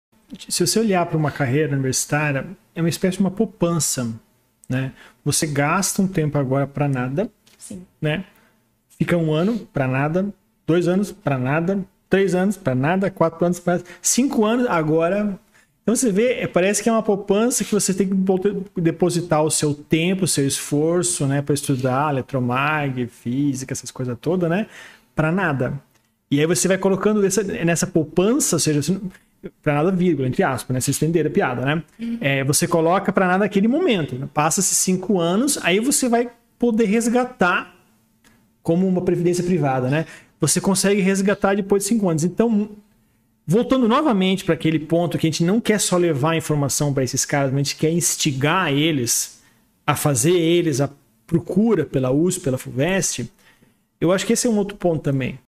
A carreira universitária é uma poupança!!!
Fazendo um curso superior  por vários anos, 3, 4 ou até 5 anos, há um investimento de tempo e esforço para estudar sem resgatar nenhum "ganho" aparente de forma imediata. Após 4 ou 5 anos investindo nesta carreira vem o retorno, vem o momento de você resgatar o investimento feito por você, com um salário mais bem remunerado. Assim, a carreira universitária é uma poupança ou como uma previdência privada que você pode resgatar o investimento somente após 4 ou 5 anos.